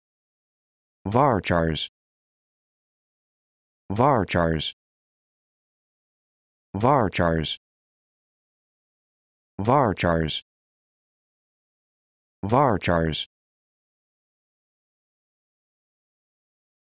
How to Pronounce Varchars
This video shows you how to pronounce Varchars